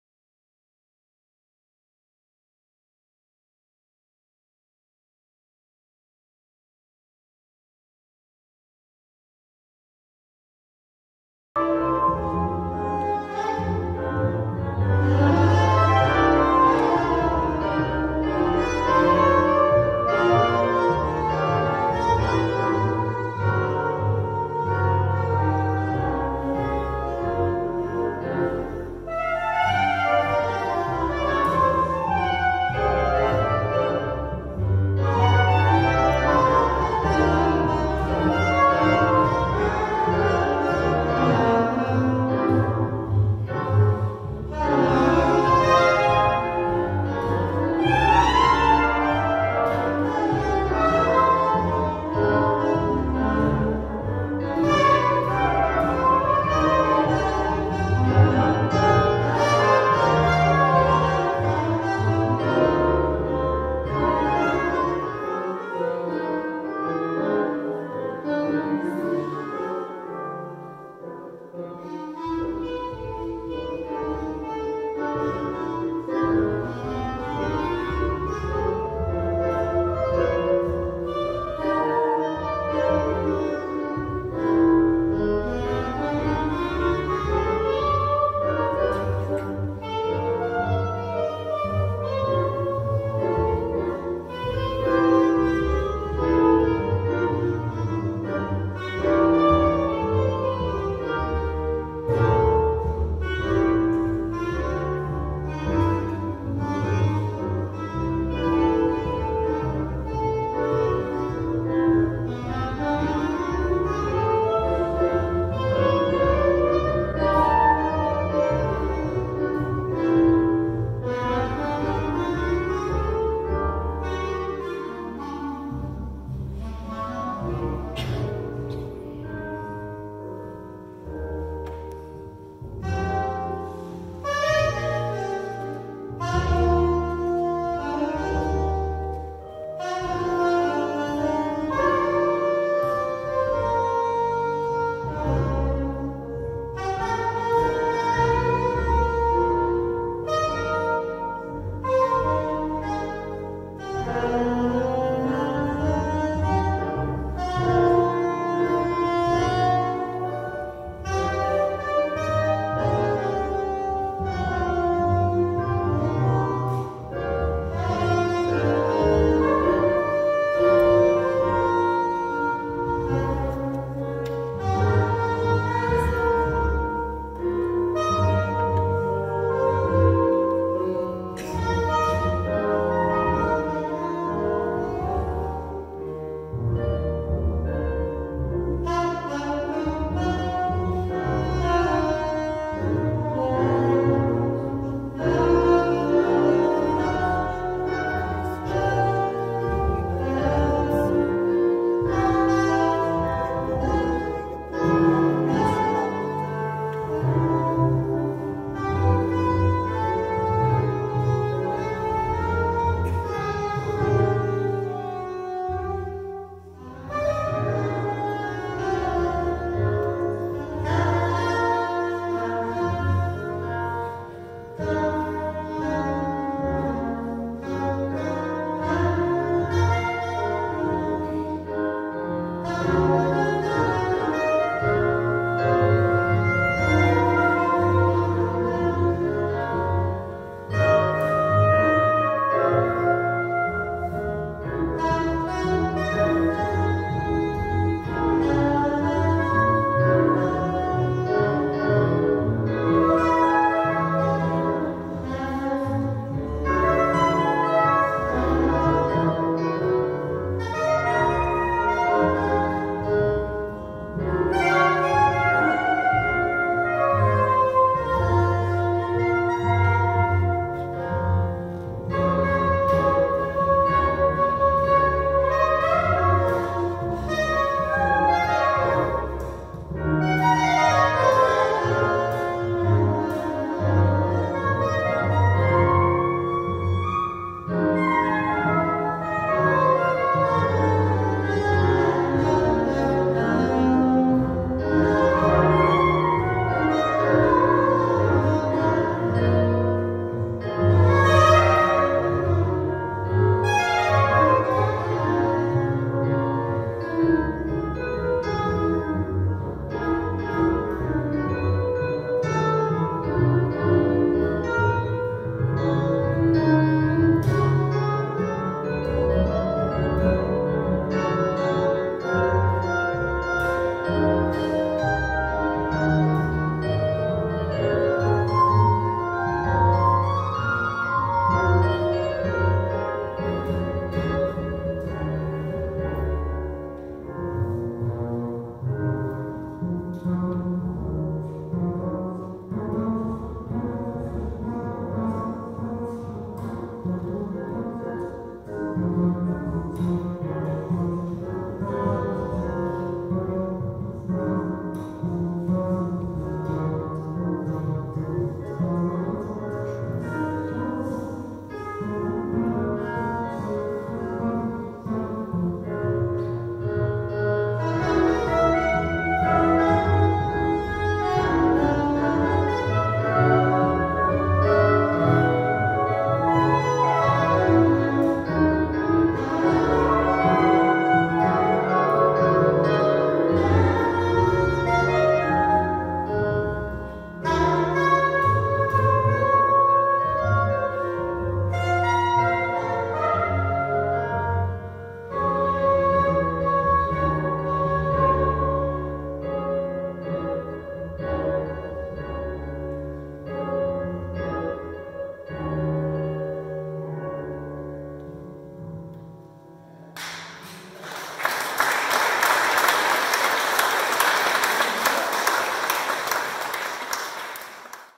Concert jazz église Ouzouer le Doyen
Soirée Jazz
Concert de Jazz dans l’église
organisé par le comité des fêtes
avec la participation du
Jazz Club de Blois